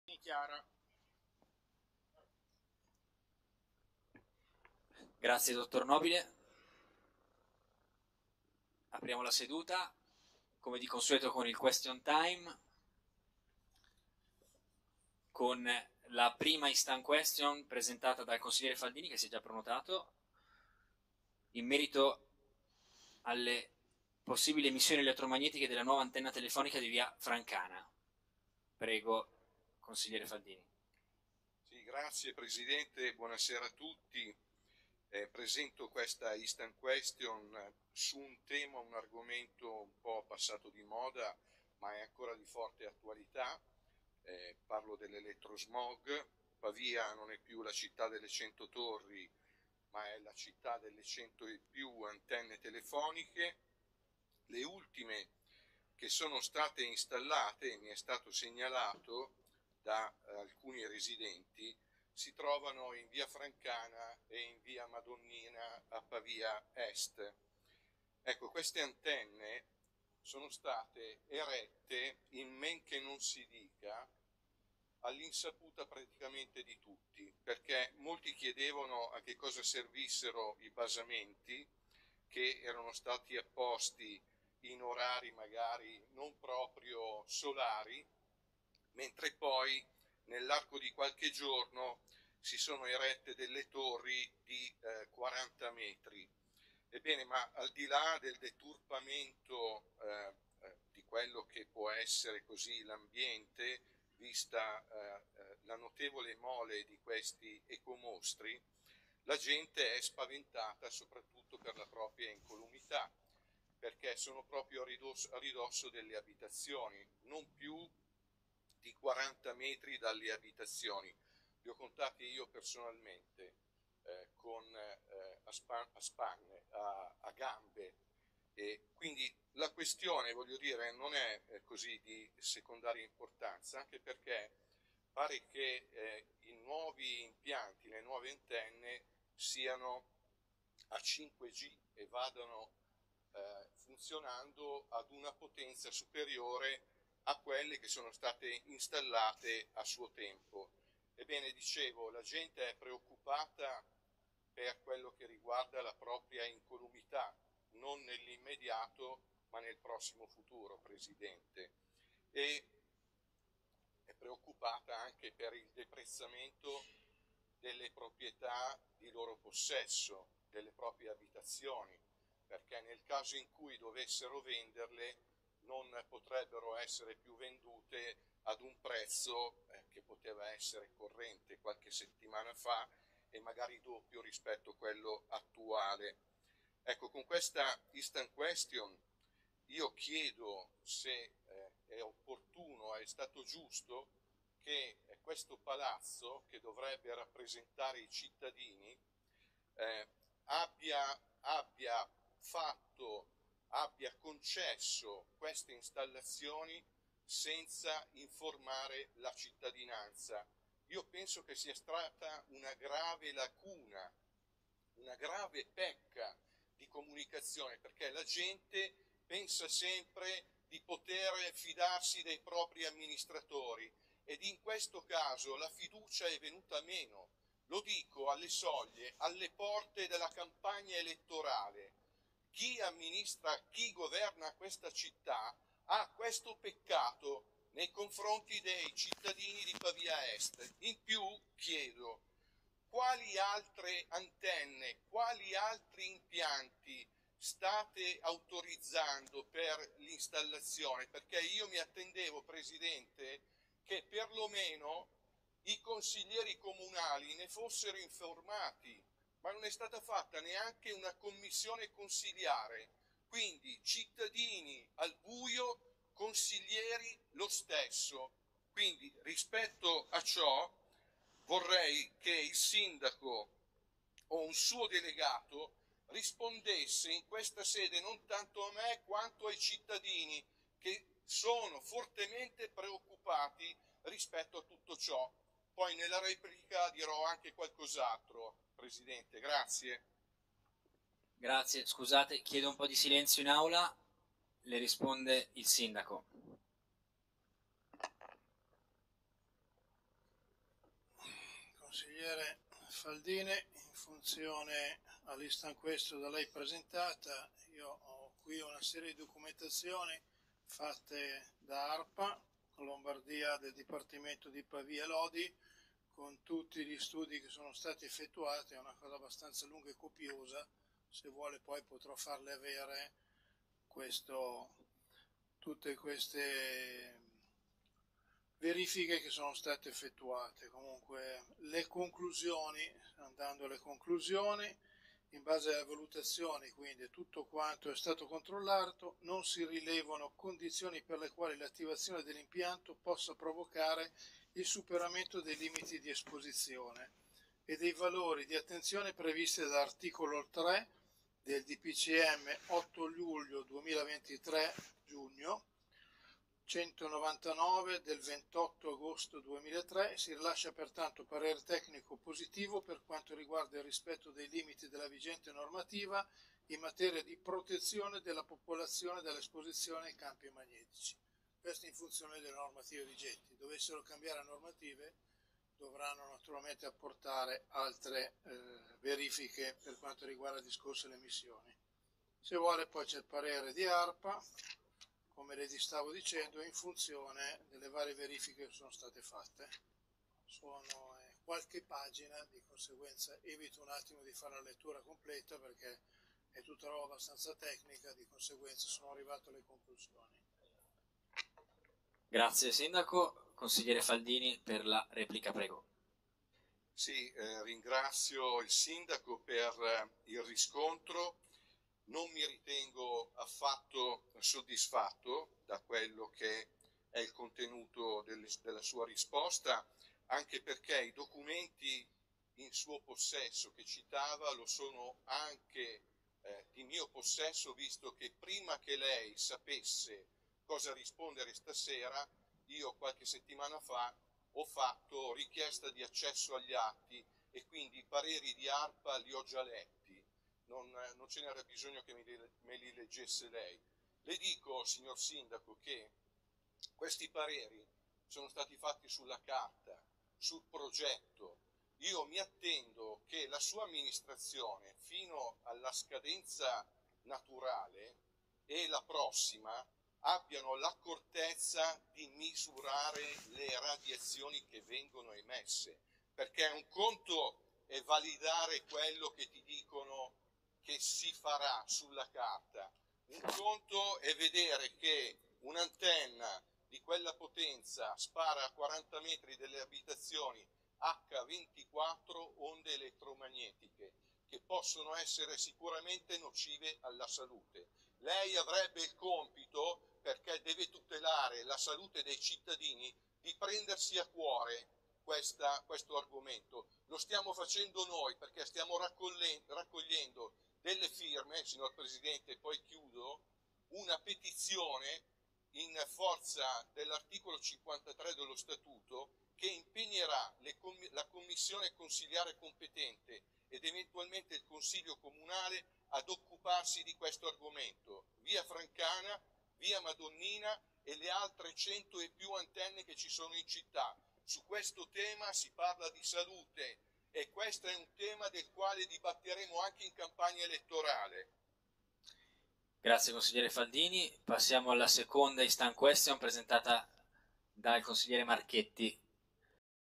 INTERROGAZIONE SU ANTENNE TELEFONICHE DI VIA FRANCANA E VIA MADONNINA. CONSIGLIO COMUNALE DEL 23.4
ANTENNE TELEFONICHE DI VIA FRANCANA E VIA MADONNINA.

Nel video si può ascoltare la mia interrogazione in aula consiliare alla quale ha risposto direttamente il sindaco. Un riscontro inutile ed insoddisfacente al quale ho risposto per le rime.

Al netto di ciò che farà prossimamente l'ormai ex primo cittadino, restano le responsabilità della sua giunta circa la mancanza di trasparenza e di comunicazione su temi importanti e sentiti come questo dell'elettrosmog.

Attenzione a chi votate il prossimo 8 e 9 giugno!